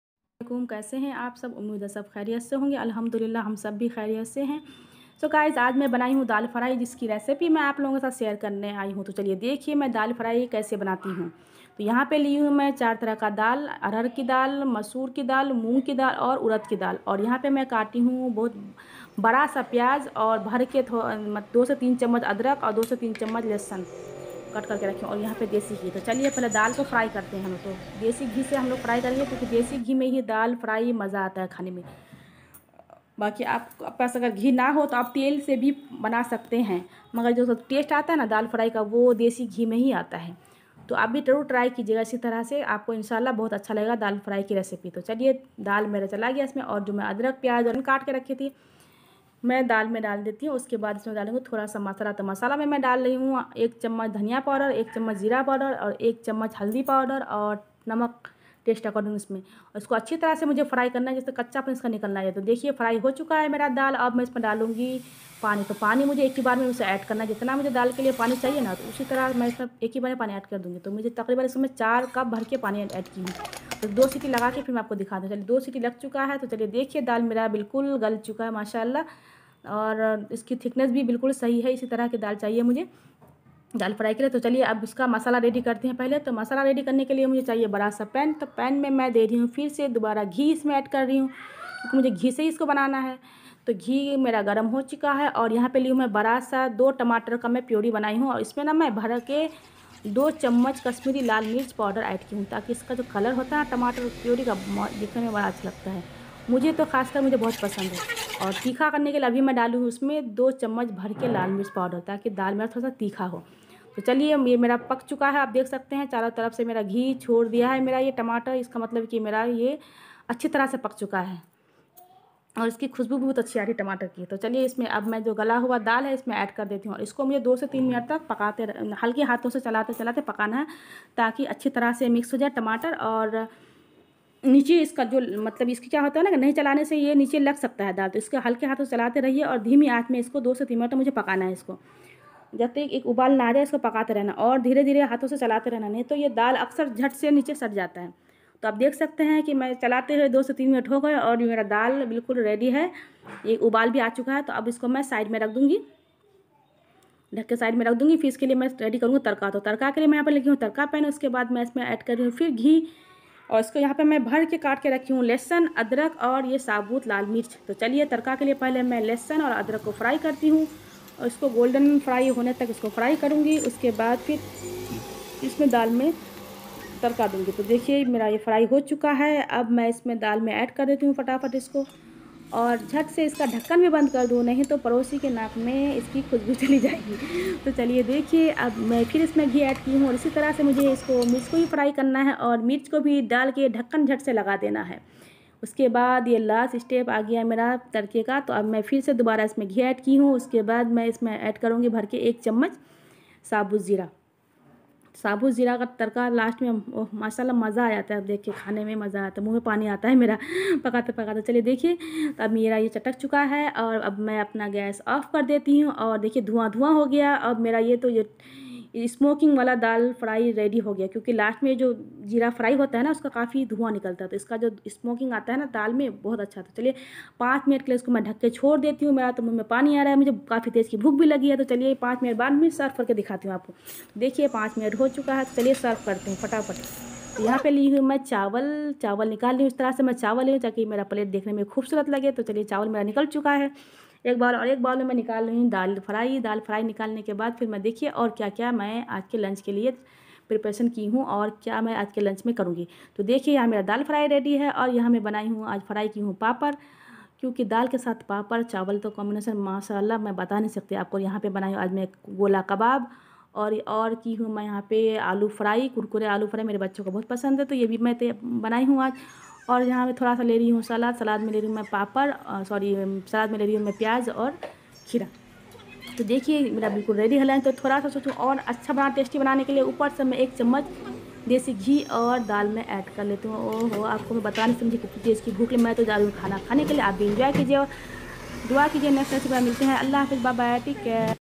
Daal fry recipe! ढाबा स्टाइल दाल फ्राई रेसिपी अब घर पर! एक बार खाए जो उंगलियाँ चाटेंगे! Niloferfoods
ingredients : for boil 1 step

4 types of dal
1)  100 gm  masoor ki daal
2) 100 gm arhar ki daal
3) 100 gm moong ki dal
4) 100 gm urad ki dal
2 teaspoon deshi ghee
1 tsp dhaniya powder
1tsp jeera powder
1tsp haldi powder
1 large size onion bareek chopped
2 tsp janjar baareek chopped
2 tsp garlic baareek chopped
namak test according
Pani 4 cups

2step
for masala tadka:

2 large size tomato puree
2 tsp kasmiri lal mirch powder
2 teaspoon deshi ghee
2 tsp laal mirch powder

3 step
for tadka:
datka for deshi ghee
2 tsp janjar baareek chopped
2 tsp garlic baareek chopped
1 tsp sabut jeera
8- 10 sabut laal mirch

is tarah se banaye daal fry insha Allah aap sbhi ko bahut acha lagega ,aur aap sb mera ye video pura dekhe aur comments kr ke jarur bataye aap ko ye daal fry ki recipe kaisi lagi, thanks for watching ❤️❤️ love you all